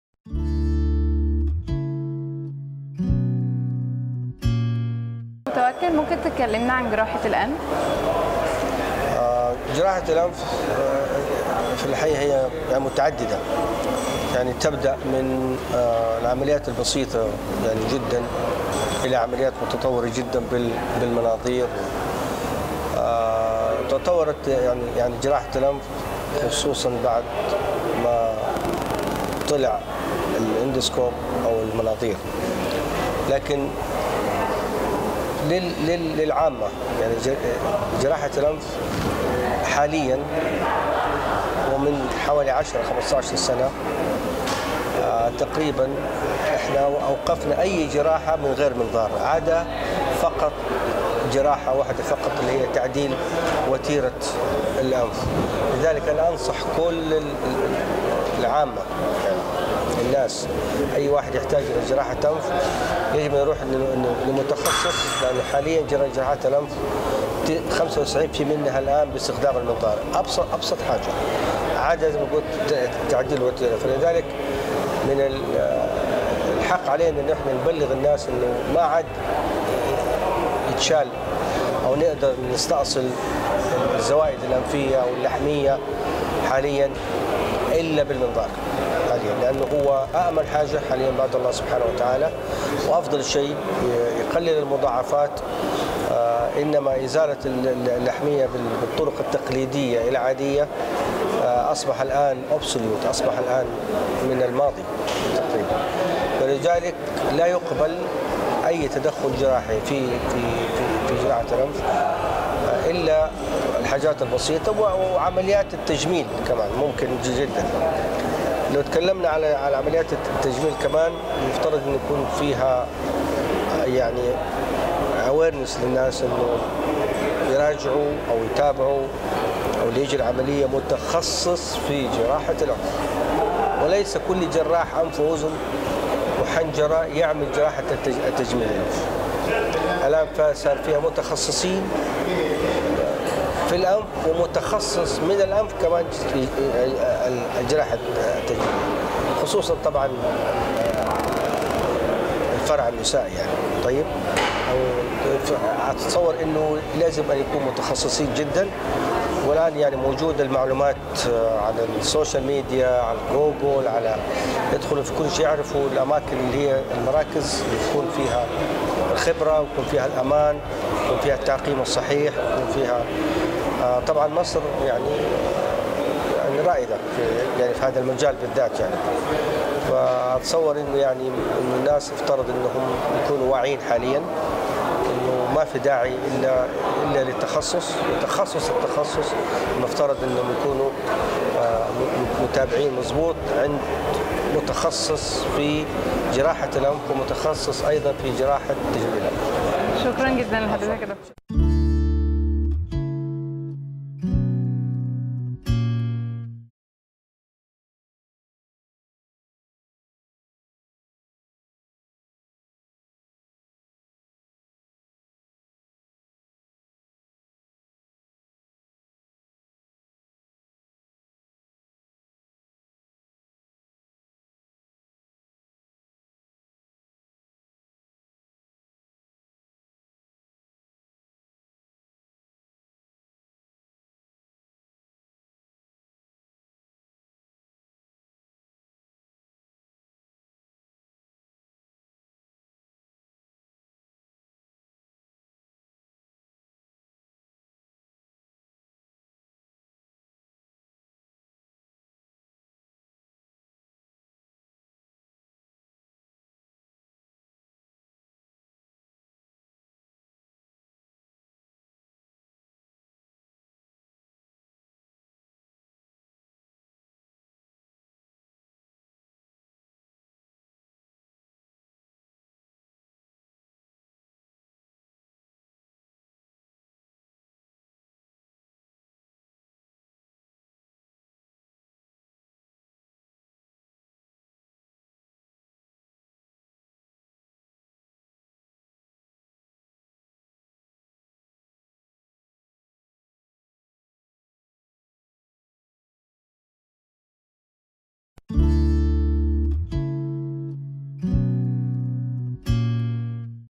تطور وتعدد جراحات الأنف / دكتور متوكل حجاج
الدكتور متوكل حجاج
استشاري انف واذن وحنجرة بمستشفى أحد بالمدينة المنورة
The Clinic